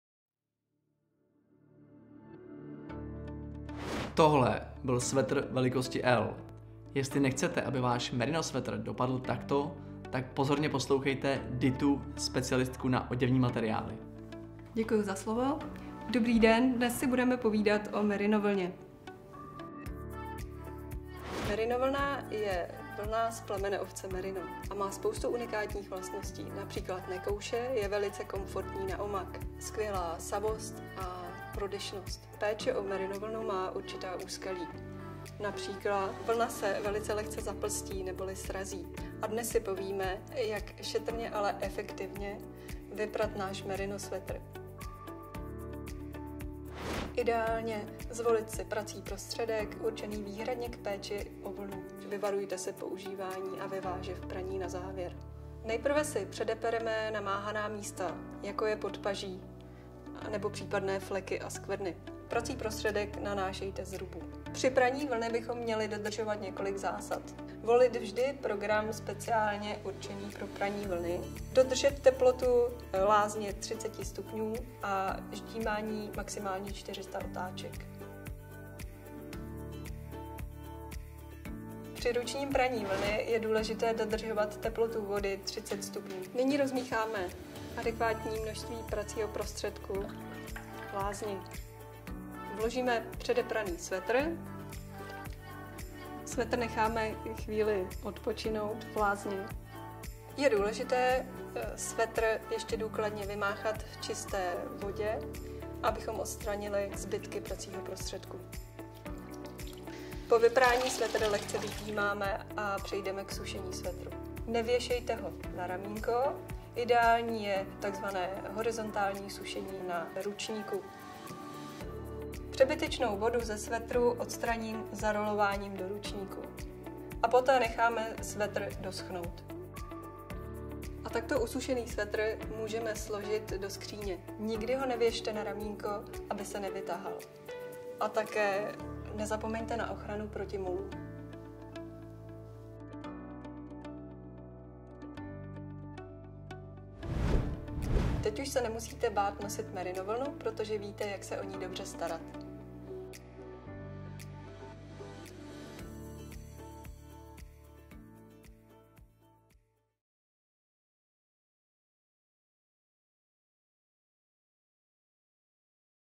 Live Sweaters: Jak pečovat o merino svetr
Podprobný návod jak vyprat merino svetr. Už se nemusíte bát si koupit merino oblečení. Dá se prát jednoduše v pračce. Tady máte přesně popsáno jak. Live Sweaters. Be the show.